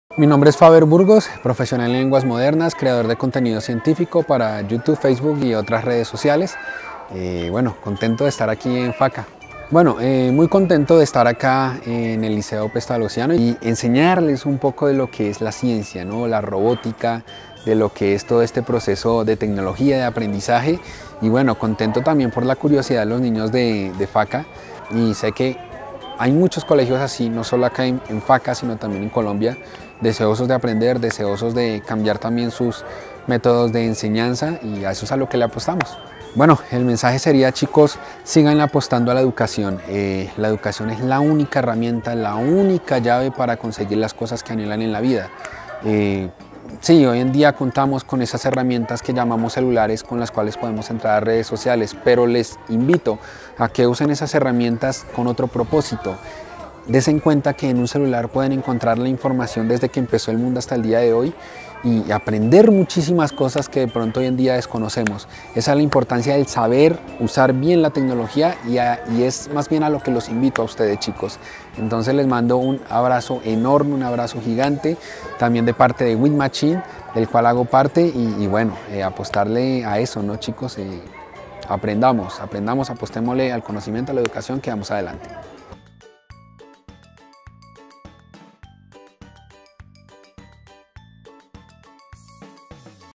Ciencia, robótica y nuevos modelos de aprendizaje para una mejor enseñanza
Educación 🧪 || Ciencia, robótica y nuevos modelos de aprendizaje para una mejor enseñanza

La Secretaría de Educación acompañó el Liceo Pestalozziano en la invitación al creador de contenido Faber Burgos, quien en su visita brindó a los estudiantes una charla en la importancia del estimular la curiosidad en el método de enseñanza.

Es así como se propone que desde la ciencia y la robótica se brinden nuevos modelos y métodos de aprendizaje para una mejor enseñanza, como clave del desarrollo personal y esta a su vez, base de una mejor sociedad.